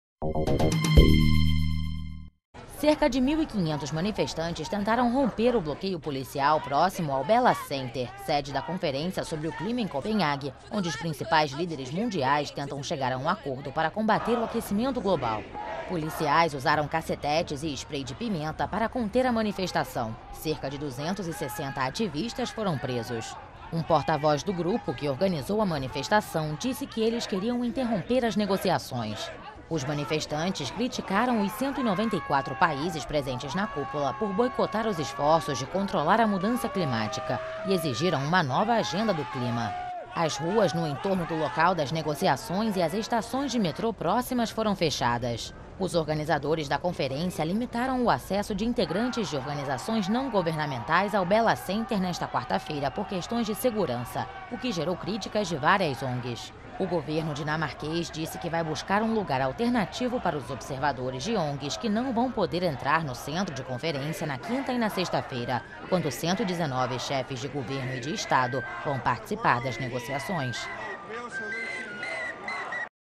Clima tenso em Copenhague
Cerca de 1.500 manifestantes tentaram nesta quarta-feira romper o bloqueio policial próximo ao Bella Center, sede da conferência sobre o clima em Copenhague (COP-15). A polícia dinamarquesa usou gás lacrimogêneo e cassetetes para conter a manifestação.